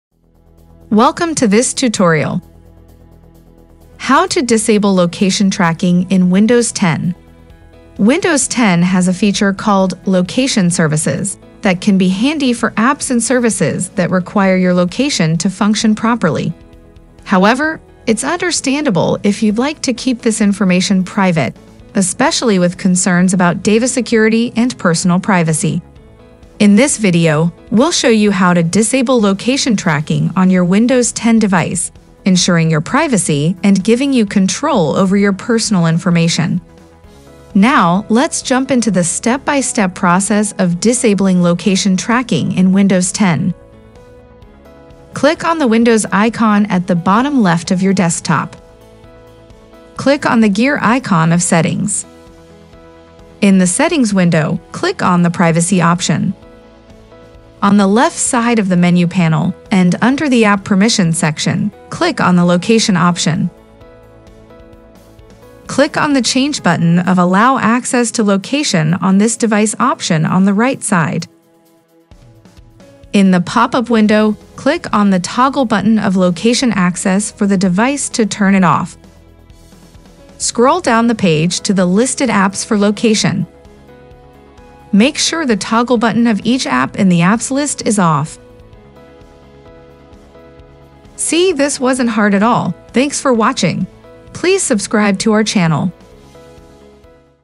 How To Disable Location Tracking In Windows 10 :Tutorial
How To Disable Location Tracking In Windows 10
Windows 10 has a feature called "Location Services" that can be handy for apps and services that require your location to function properly. However, it's understandable if you'd like to keep this information private, especially with concerns about data security and personal privacy.
In this video, we'll show you how to disable location tracking on your Windows 10 device, ensuring your privacy and giving you control over your personal information. Now, let's jump into the step-by-step process of disabling location tracking in Windows 10.
1. Click on the Windows icon at the bottom left of your desktop.

2. Click on the gear icon of Settings.

3. In the Settings window, click on the Privacy option.

4. On the left side of the menu panel, and under the App permissions section, click on the Location option.

5. Click on the Change button of Allow access to location on this device option on the right side.

6. In the pop-up window, Click on the toggle button of location access for the device to turn it off.

7. Scroll down the page to the listed apps for location.

8. Make sure the toggle button of each app in the apps list is Off. If not then turn it off.